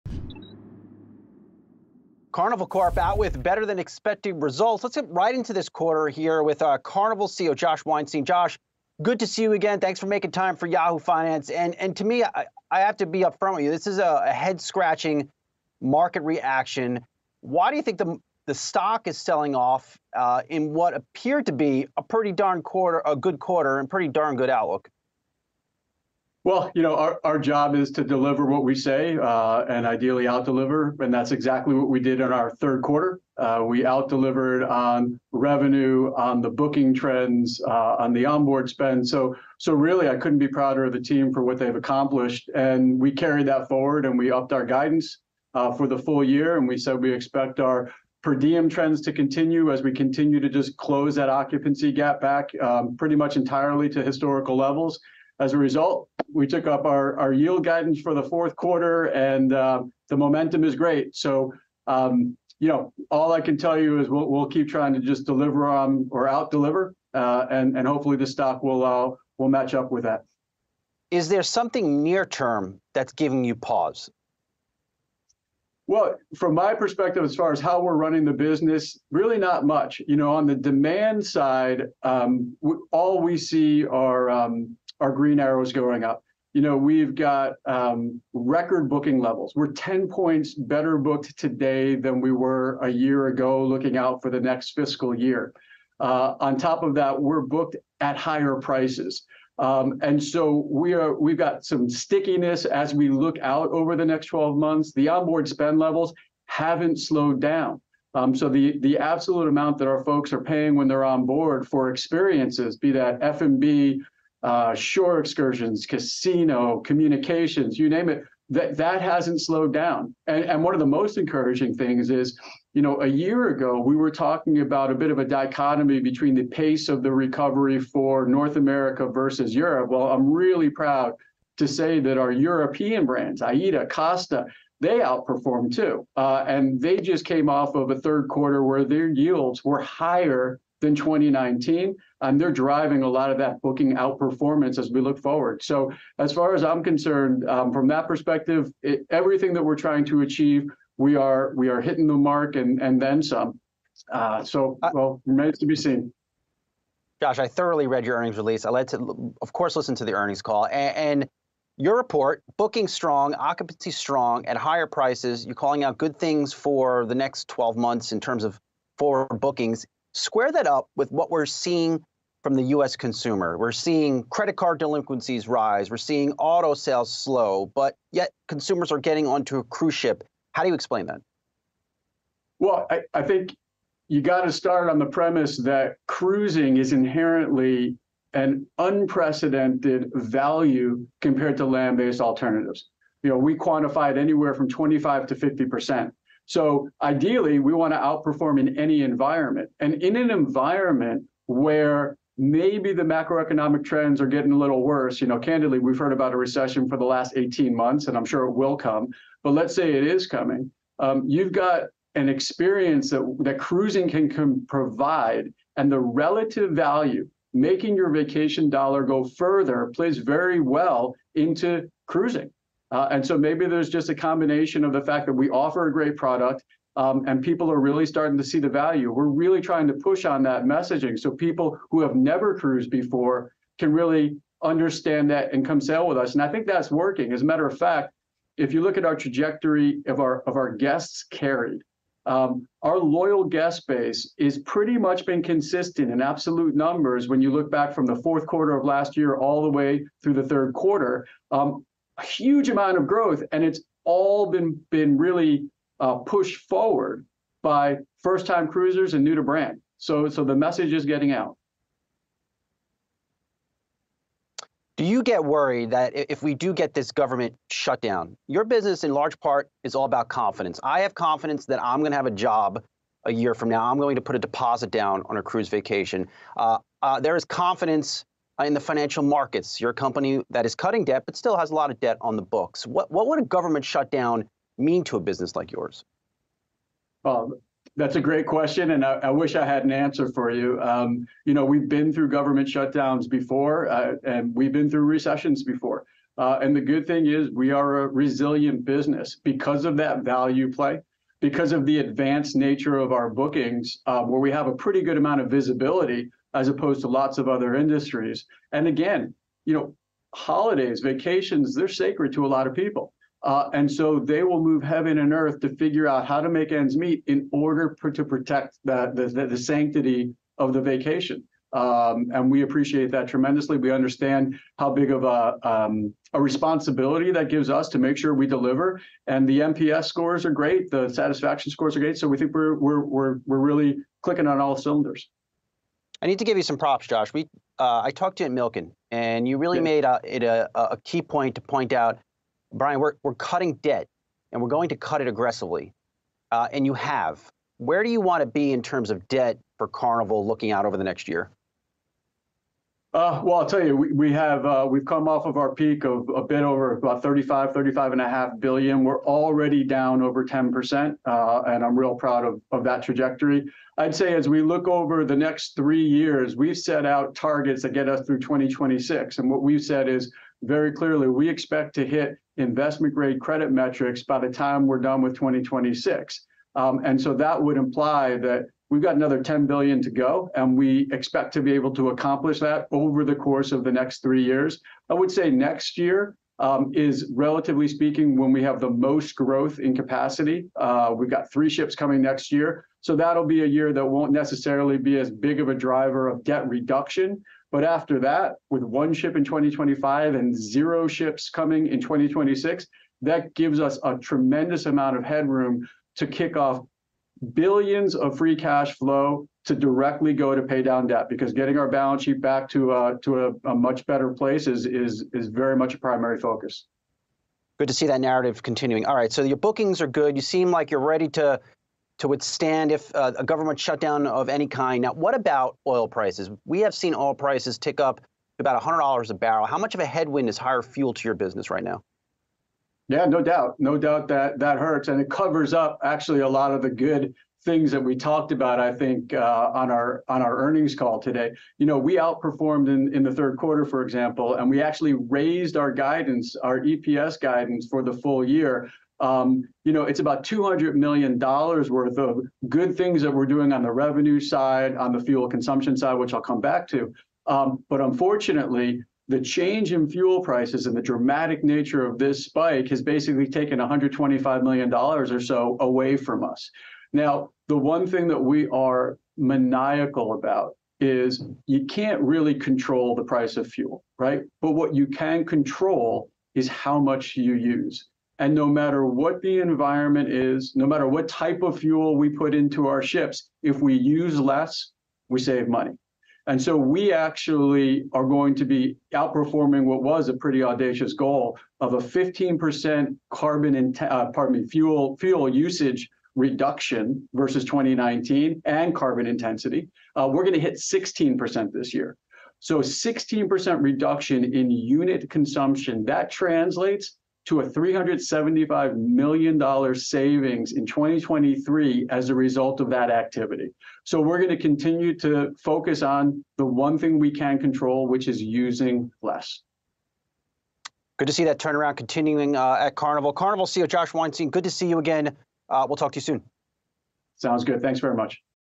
Carnival beats in earning estimates, stock slides: CEO discusses demand, debt, and fuel costs
Shares of Carnival (CCL) closed lower on Friday despite reporting record third-quarter revenues of $6.9 billion. Carnival CEO Josh Weinstein joins Yahoo Finance Executive Editor Brian Sozzi to break down why the company isn't concerned with the stock price and has a positive outlook going forward. Weinstein boasted the company's strong occupancy with bookings at higher prices. "Cruising is inherently an unprecedented value compared to land-based alternatives," Weinstein said, pointing out why consumers are willing to spend money on a cruise vacation despite recent economic headwinds. But travelers aren't just booking vacations. They are spending more when they get on board too. "As we look out over the next 12 months, the onboard spend levels haven't slowed down, the absolute amount that folks are paying for when they're on board for experiences...hasn't slowed down," Weinstein said.